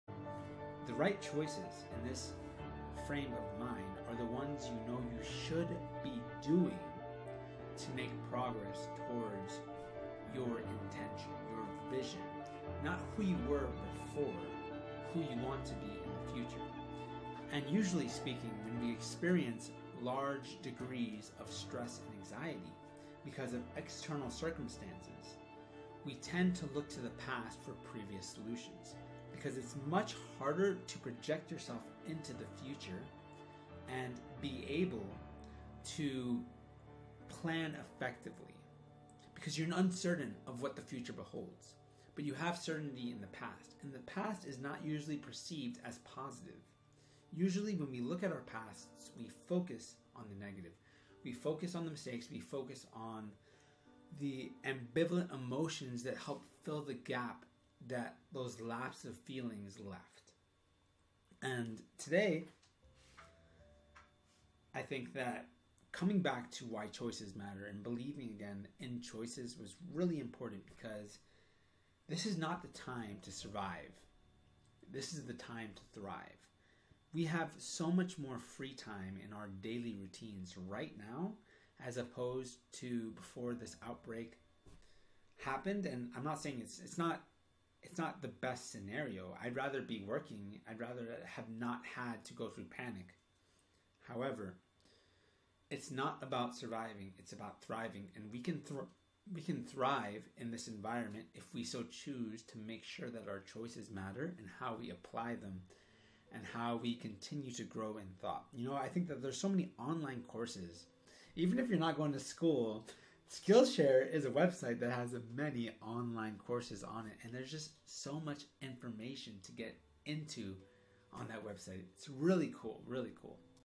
Lets thrive not survive :D
Of course surviving is good to, but I dont think yall realize just how much free time you now have on your hands to do all those things you put off because you didn't have time in the first place.

Take quarantine seriously or it will not take you seriously at all!